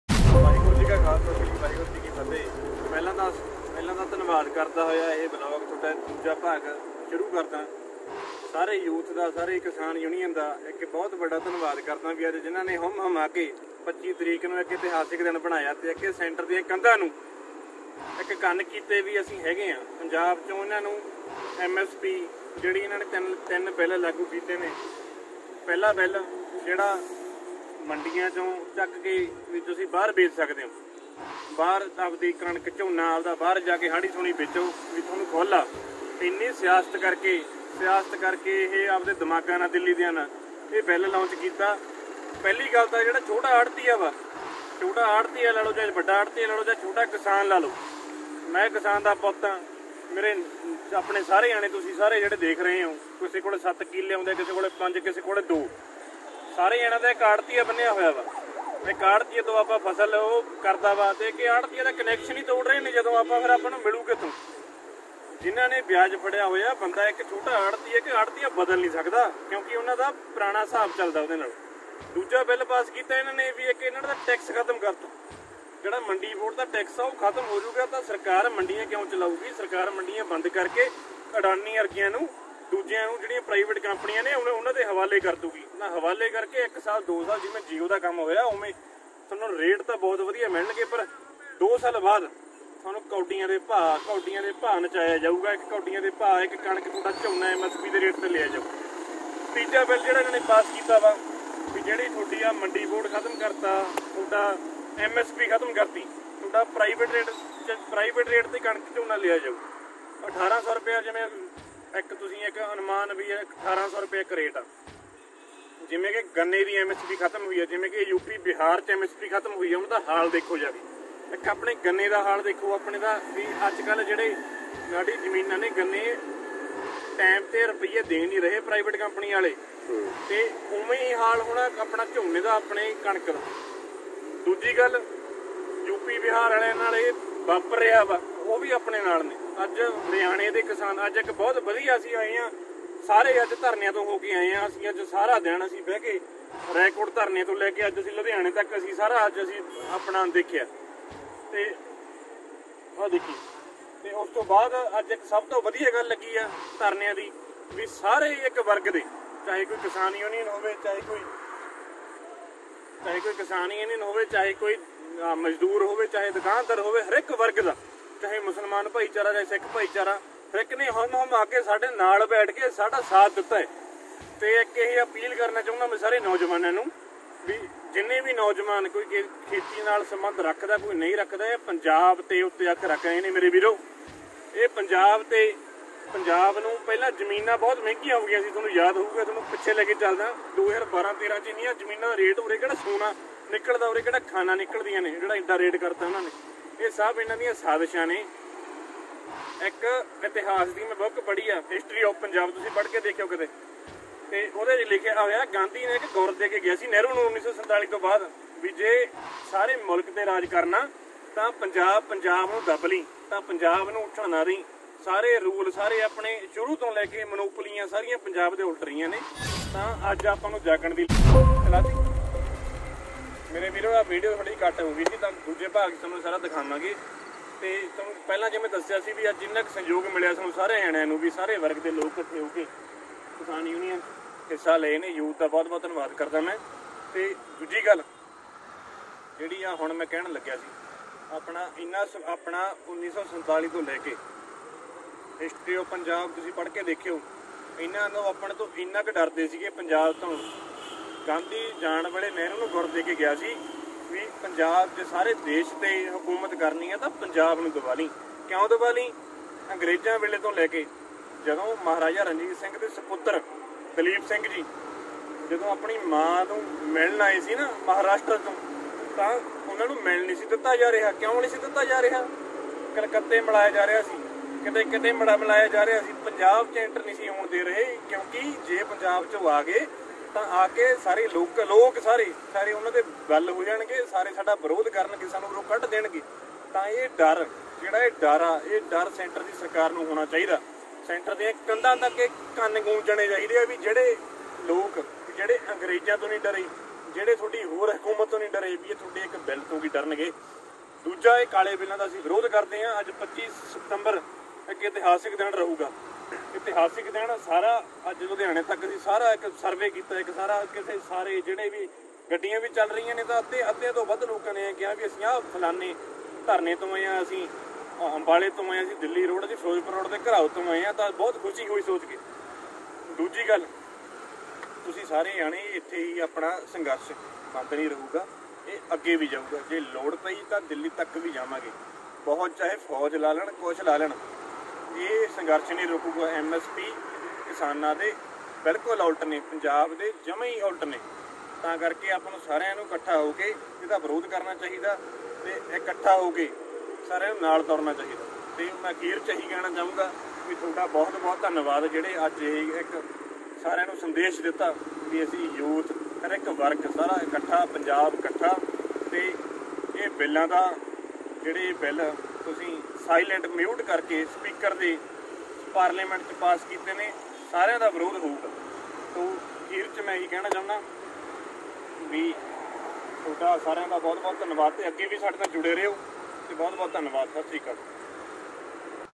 STUDENTS PROTEST AGAINST BLACK BILLS (MSP)|| BHARAT BANDH || SUPPORT FARMERS || KISSAN EKTA ZINDABAD
Punjab farmers on Friday took to the streets and held protests at 125 sites against the three farm Bills cleared by Parliament, which they dubbed as “black laws”. They blocked roads, including highways, to press the Centre for the withdrawal of the legislations, which are yet to get the President’s approval.

jehde sadi video dekh rahe ne sareya da teh dilo dhanwaad . aao sare aapan kathe hoke dehli nu pehraav kariye te borders block kriye .eh jang jari rahu udo tak jdo tak kaale bill vapis ne lai jande . te jehde v sayasi bande ya samaj sewa dal de aagu ya singers aaye ne ohna sareya da dhanwaad .

umeed karda tusi aage v sade nal jude raho ga ./\/\/\.